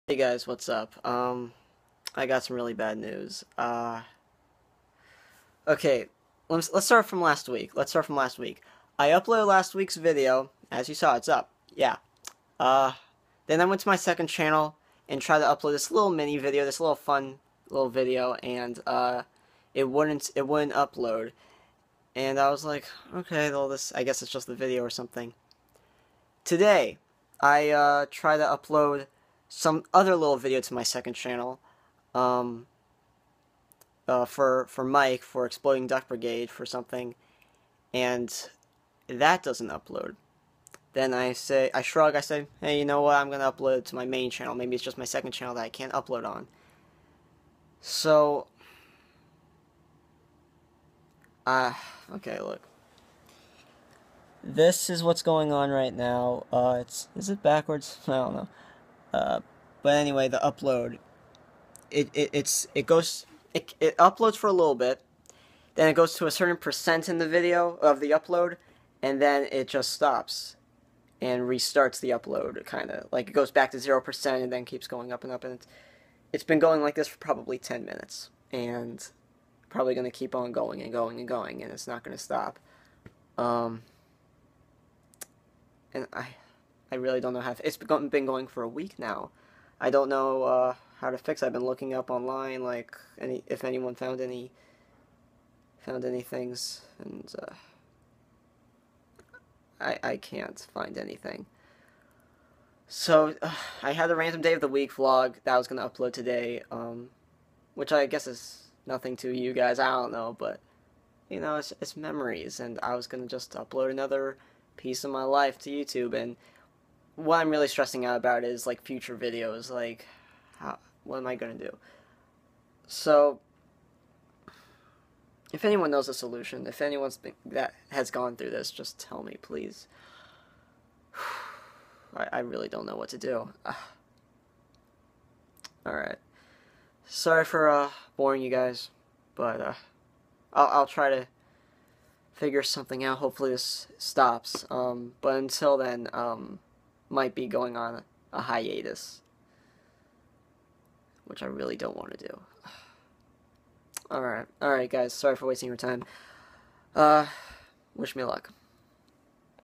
Can't Upload Videos (iPhone Vlog #1)
Might be going on a hiatus. Don't know how long this issue will last. If anyone can help in any way, please do. It helps out a ton.